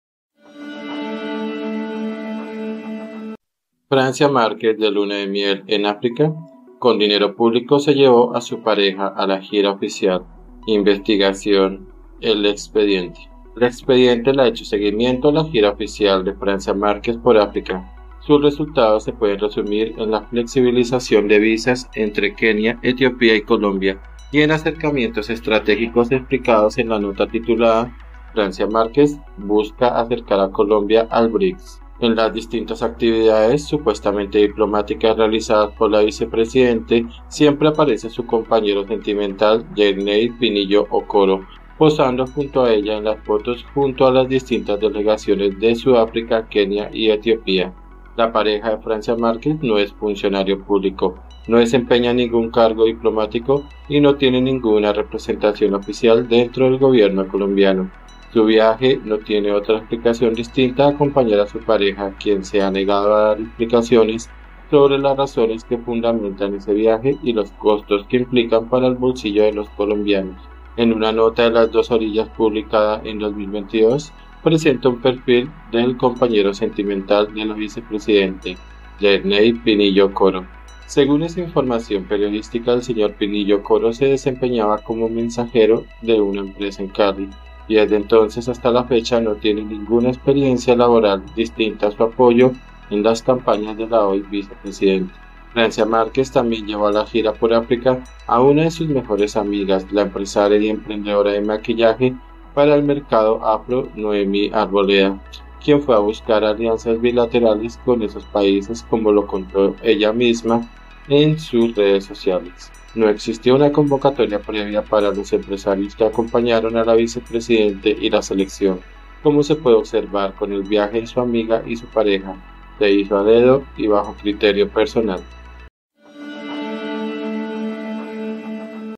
🎯 ¿FRANCIA MÁRQUEZ DE LUNA DE MIEL EN AFRICA? Por El Expediente
El Expediente le ha hecho seguimiento a la gira oficial de Francia Márquez por África. Sus resultados se pueden resumir en la flexibilización de visas entre Kenia, Etiopía y Colombia y en acercamientos estratégicos explicados  en la nota titulada Francia Márquez busca acercar a Colombia al BRICS.
Fuente: El Expediente